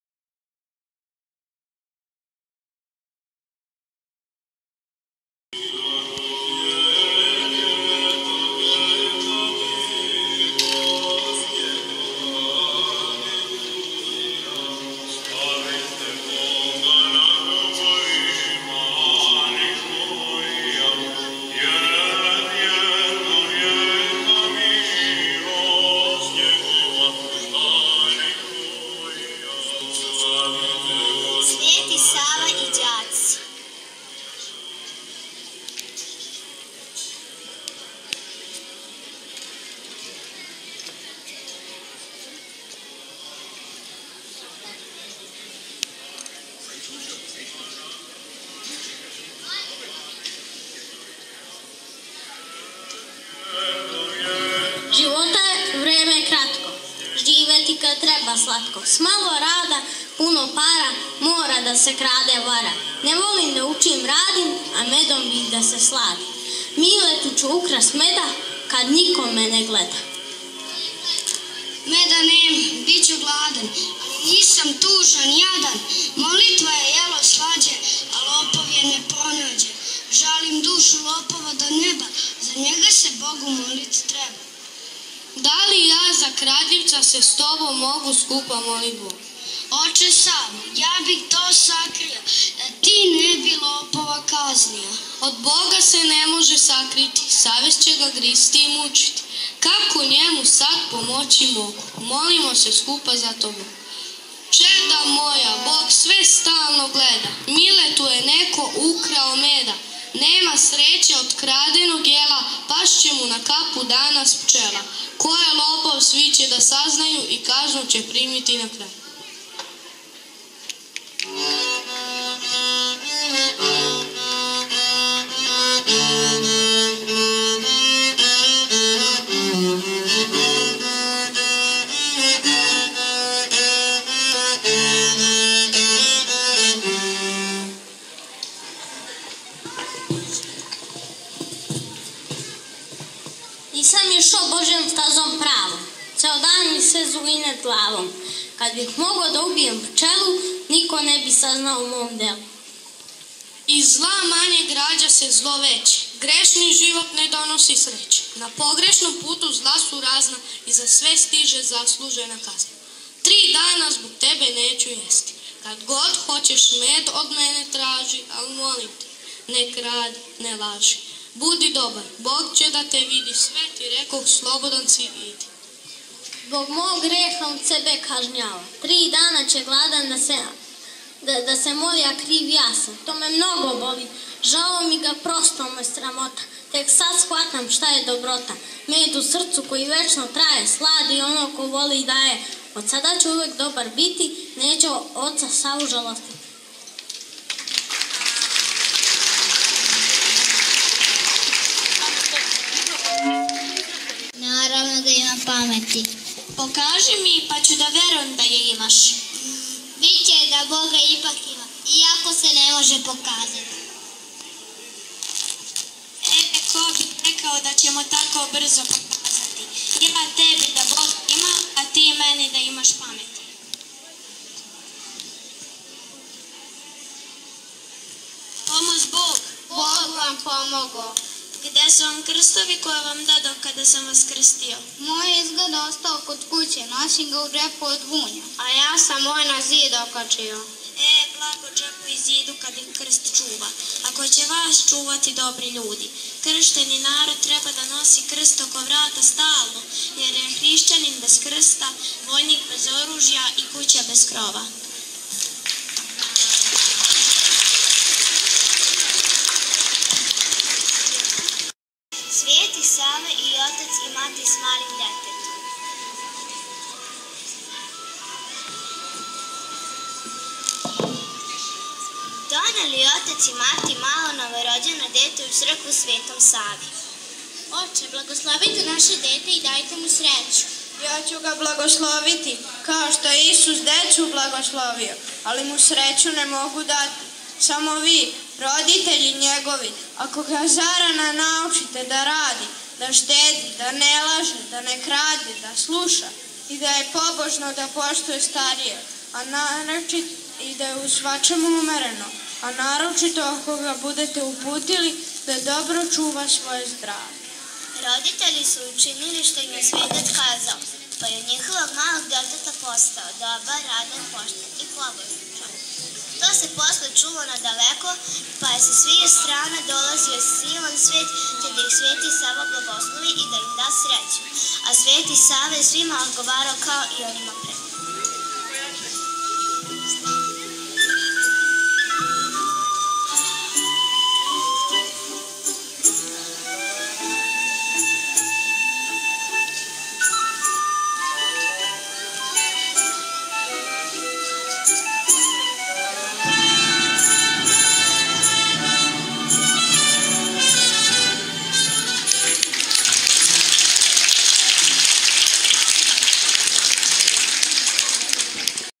II6 Колаж прича о Светом Сави 2016.
На основу прича о Светом Сави из часописа Светосавско звонце настао је овај сценски приказ у част и славу Светог Саве. Драматизовали смо текстове ,,Свети Сава и ђаци",  ,,Има ли Бога?", ,, Свети Сава и два брата" и за крај ,, Свети Сава и отац и мати са малим дететом". Учествовало је цело одељење. Снимке направио родитељ Срђан Кососвац